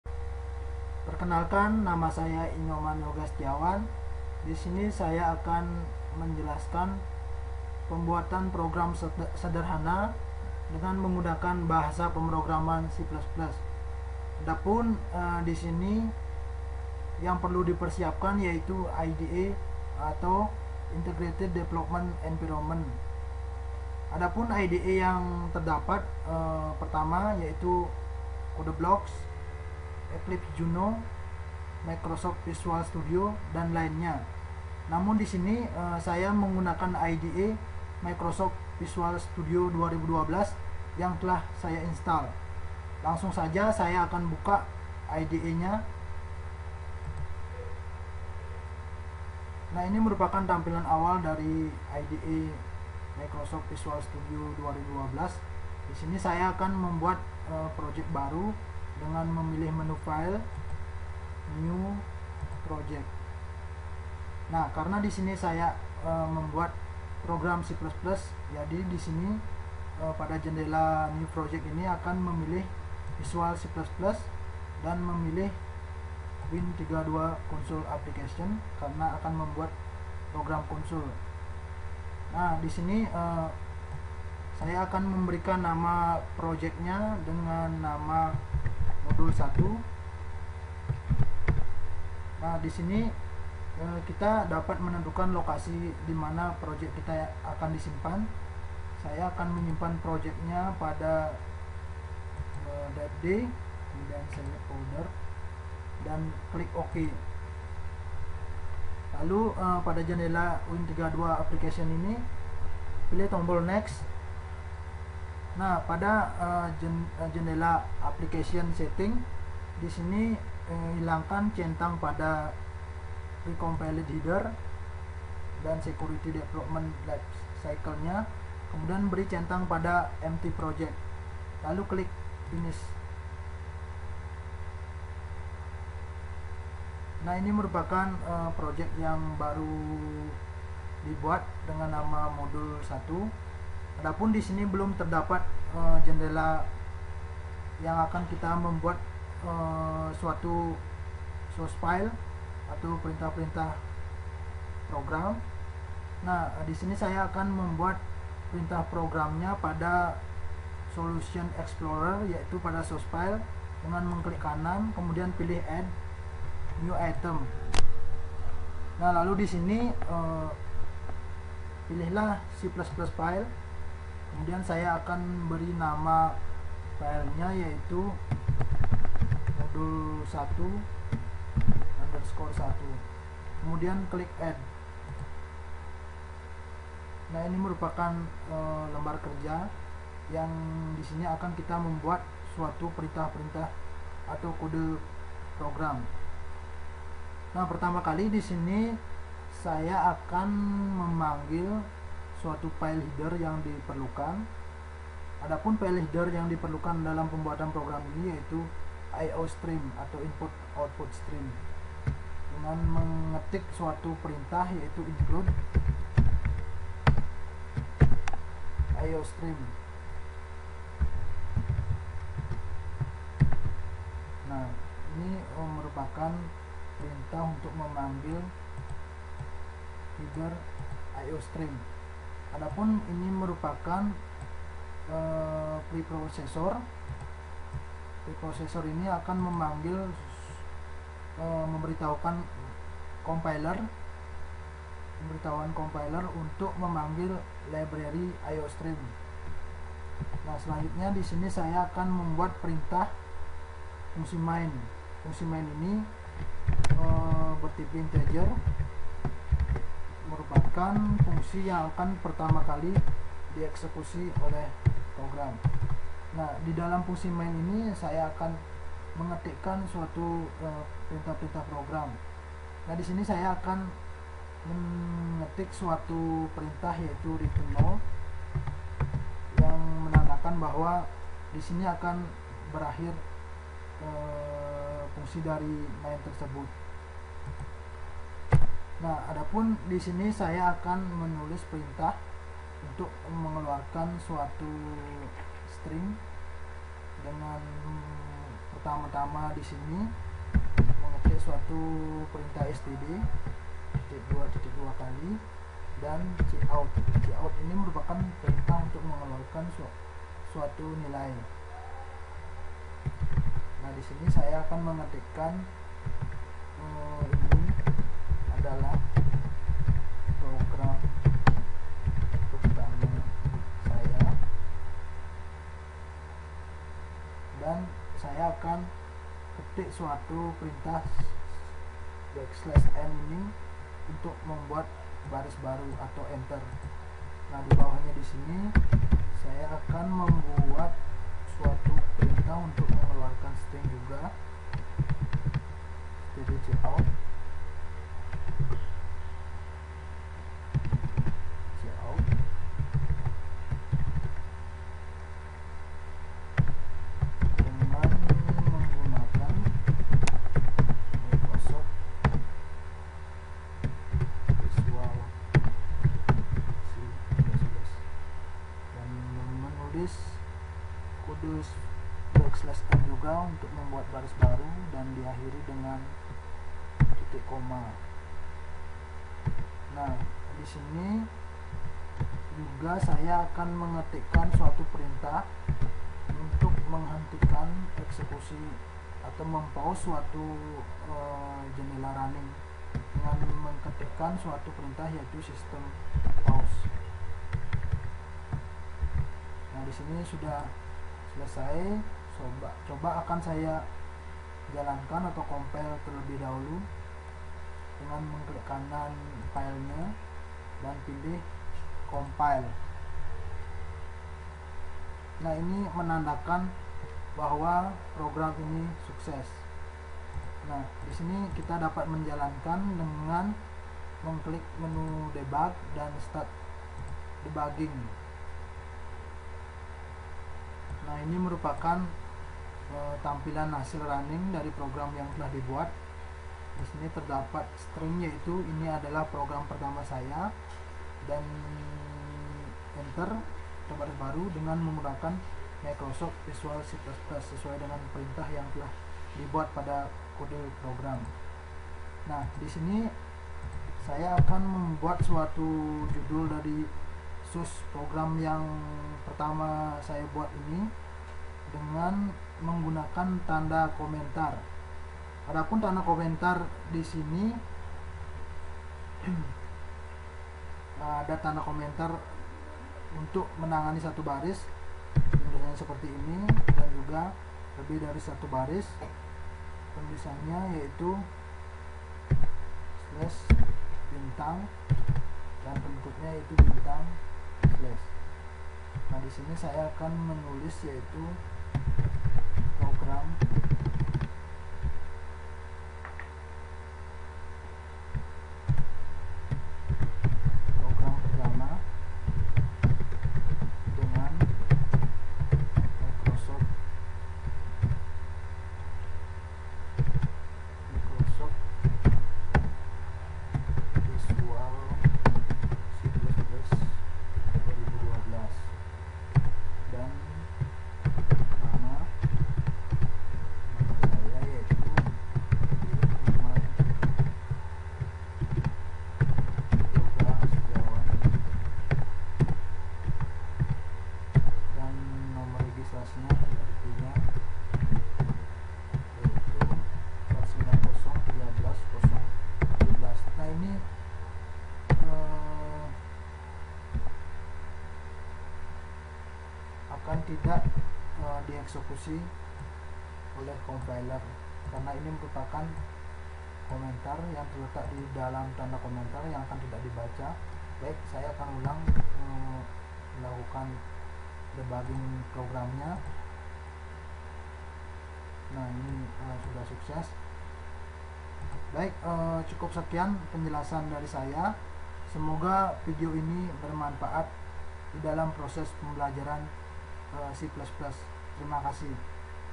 Dasar-Dasar Pemrograman C++
Video ini merupakan video tutorial pembuatan program dasar yang menampilkan keluaran(output) ke layar monitor pada pemrograman C++ dengan menggunakan IDE Microsoft Visual Studio 2012